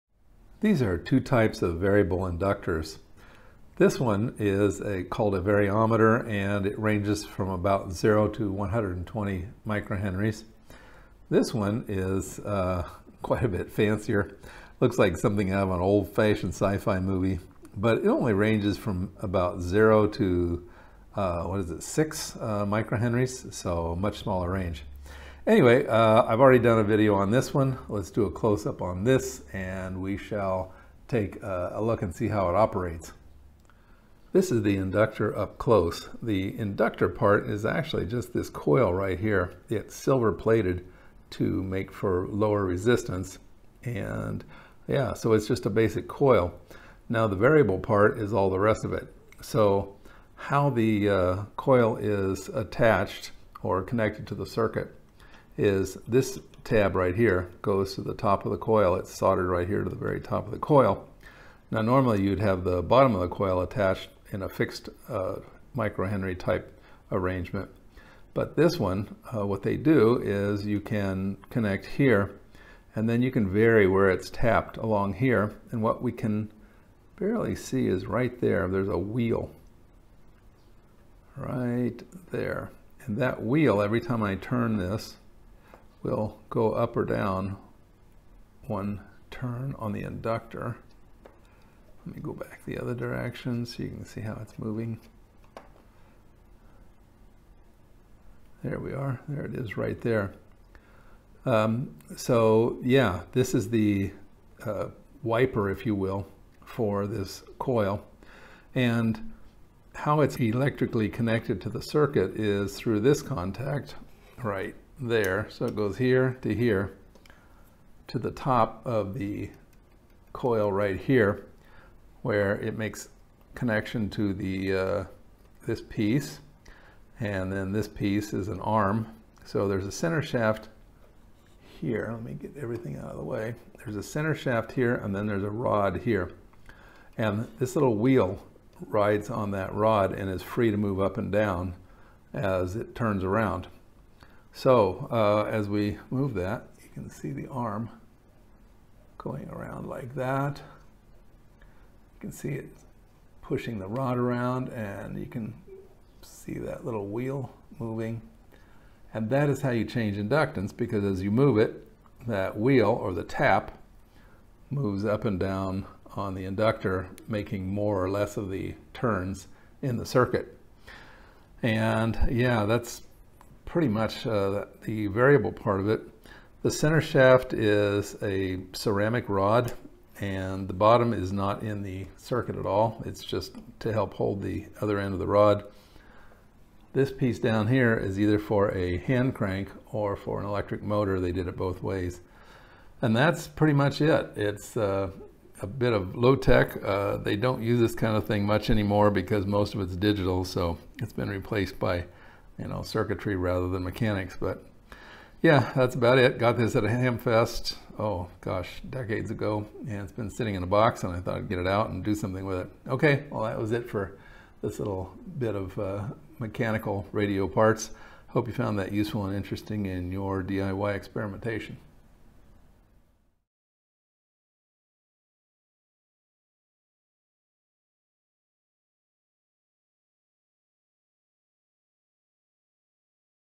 Crystal Radio--Spiral Variable Inductor (4K)
A quick overview of an different style of variable inductor.
This one has larger windings that can carry a higher current
for transmitting. The windings are silver plated for low noise
and low resistance. They were both hand cranked and motor driven.